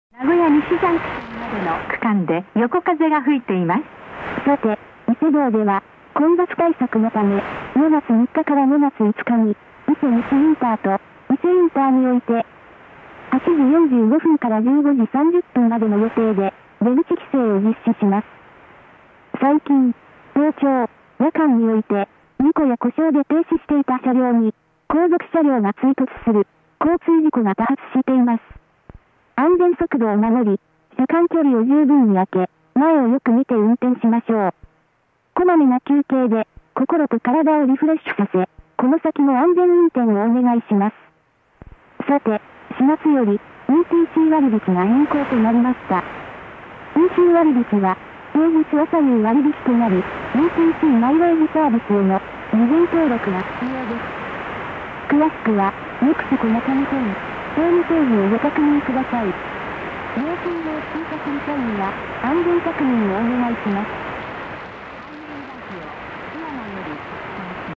[分かる方!] ハイウェイラジオ 不明 2014年5月 0時48分現在 [NEXCO中日本]
本録音は、前後関係から以下の★の位置で録音されたものだと思われる。
(名神)→瀬田→草津→(新名神)→金勝山トンネル(上り)→甲南トンネル(上り)→甲賀→★→(東名阪)→鈴鹿→四日市→(伊勢湾岸)→朝日→湾岸桑名→名港潮見
となると、「ハイウェイラジオ 土山」だと思われるが、01:04を聴くと「土山」とは聞こえない...

録音機材：PJ-30(オリンパス)
1620kHzの受信および録音(mp3)が可能

録音に関してはPJ-35も同等。

[関連記事]

PJ-30(オリンパスのワンセグ音声対応ラジオレコーダー)を入手(レビュー)

オリンパスのポータブル型ラジオ録音機 PJ-30 を使用中 (メモリー管理と音質設定)